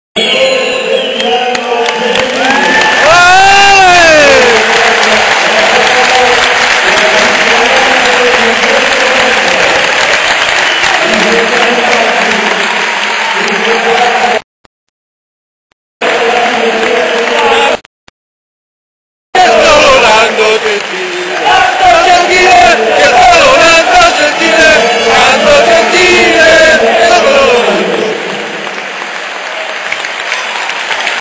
Eldo Juvecaserta-Lottomatica Roma 26.1.2008 presentazione Nando Gentile
Il Capitano torna nel Tempio...